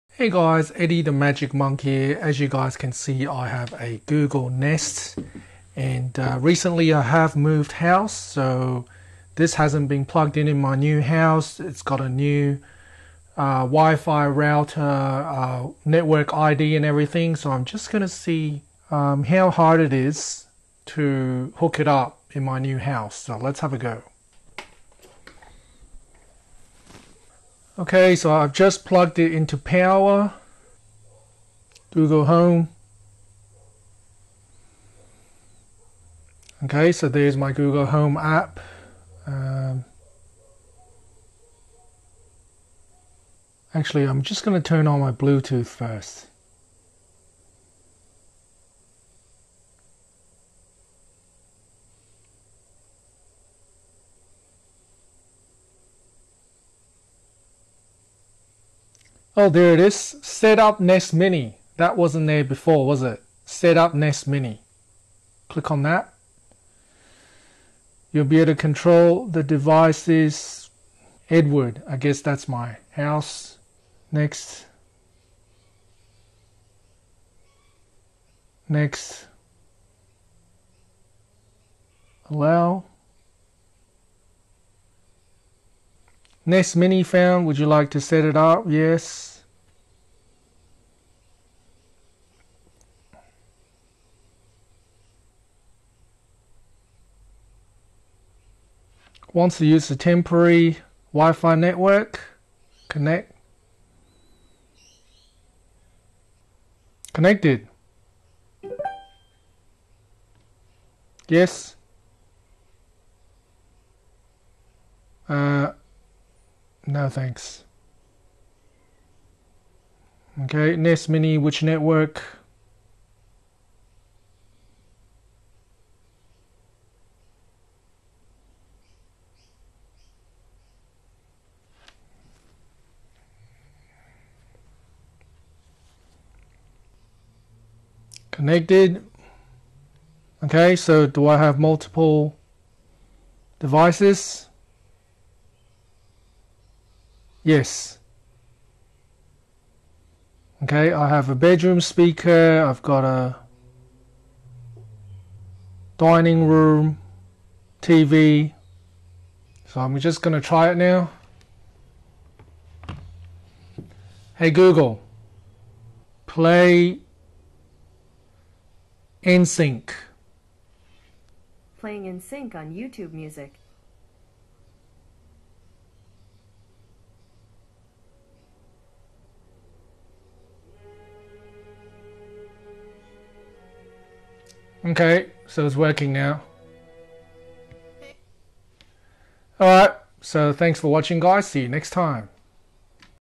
Google Nest Mini - How to connect to a different WIFI after moving house
In my old house I used to have a Google Nest Mini in my bedroom. I didn't do much with it except if I wanted to listen to music, I would just say "Hey Google, Play ###". I moved house about a year ago and I have not connected the Nest Mini to my new WIFI until today. The reason why I delayed it for so long was because there was just so much to do after I moved house, and I didn't have a clear picture in my head how I would reconnect the Nest Mini (it doesn't really have any buttons on it). Today I decided to bite the bullet and figure it out at the same time as recording the video. I have added some prompts in the video to make it simpler to follow. The main steps basically include using the Google Home app to try and detect the Nest Mini (even though the Nest Mini is not currently connected to the Home WIFI, as long as it is turned on, there are ways for your phone to detect it), and make it connect to the home WIFI again. Watch the video if you ever run into the same problem!